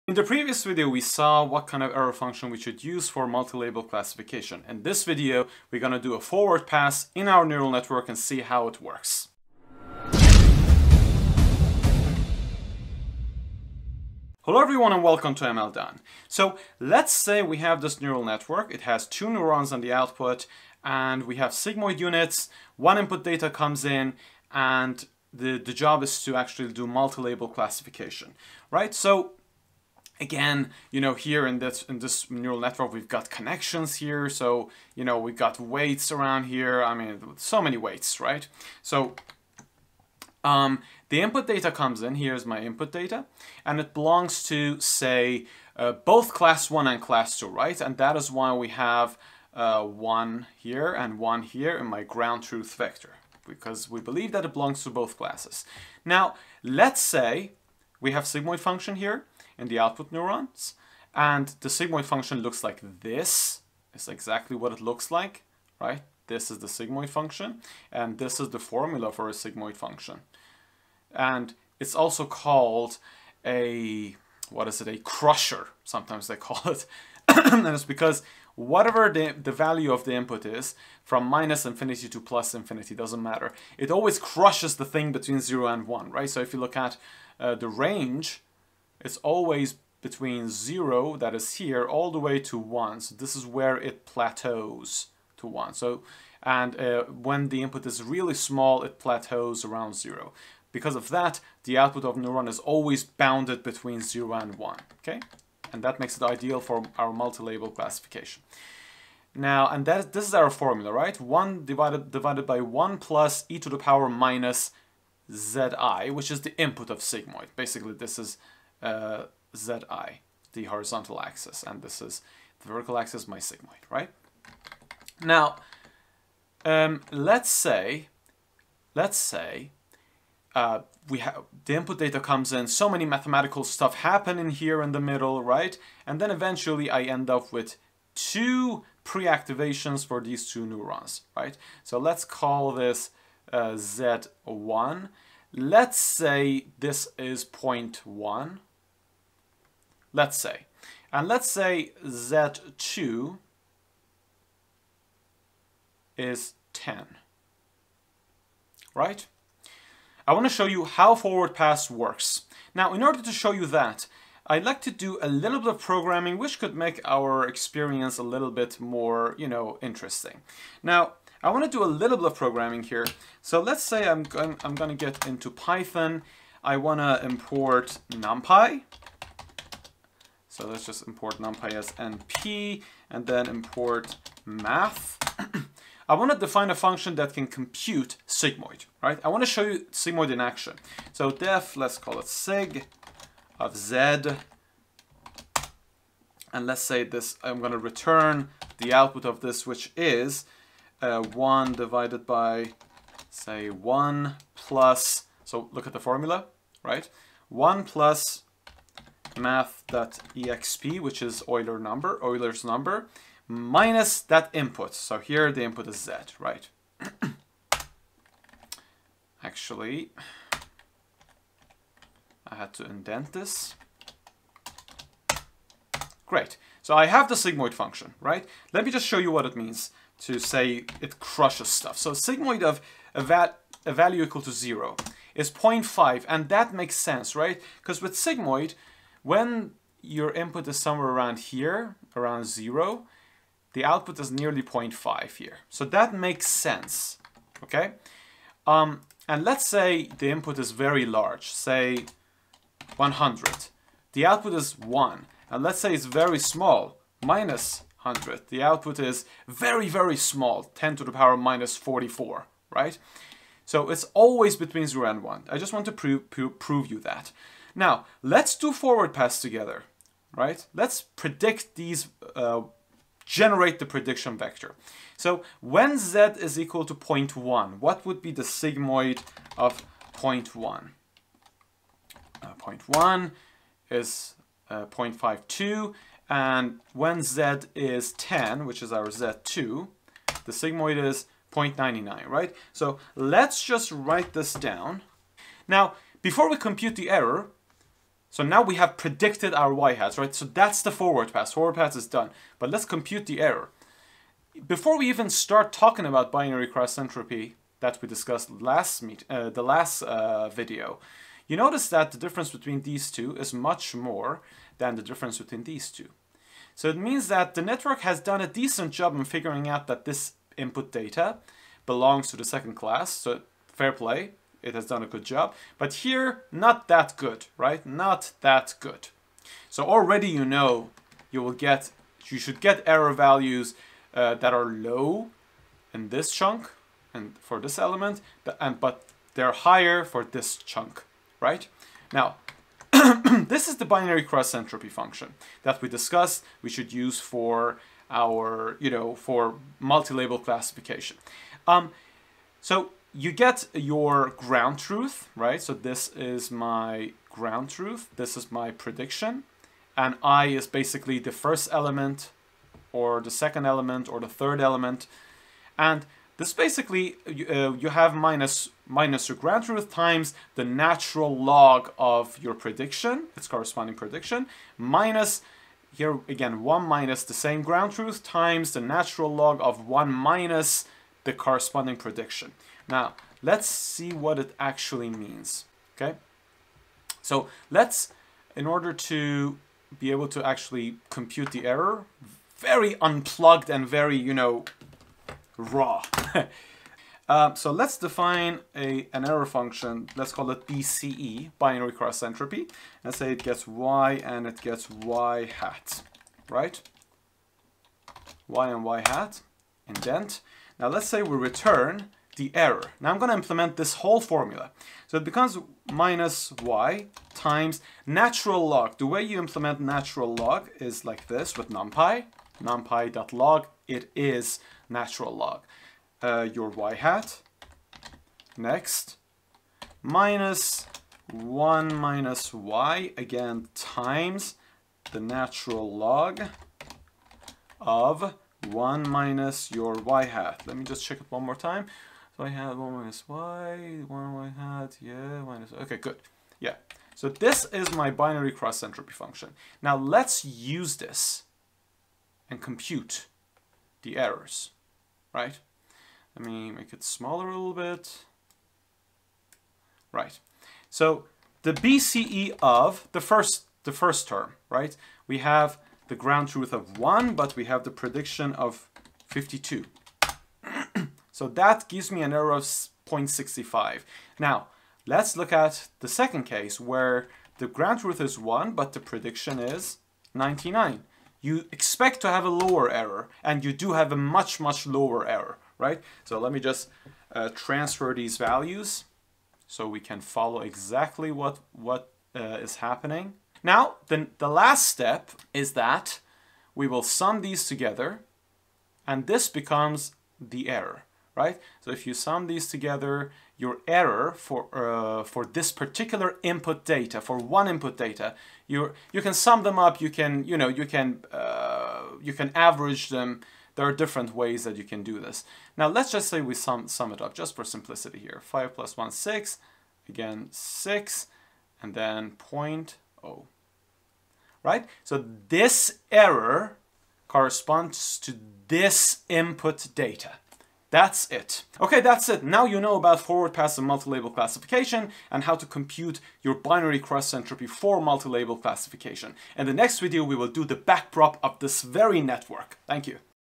Multi Label Classification and Forward Pass
📢📢📢In Multi Label Classification we can do Forward Pass and Backward Pass in an Artificial Neural Network. In this video, the foprmer is dicussed with a little bit of coding in Python.
00:00 Intro
00:45 Setting up a Multi Label classification scenario
00:56 Remembering the beauty of Sigmoid function for Multi Label Classification
02:06 Forward Pass for Multi Label Classification begins!!!
08:51 Wrap up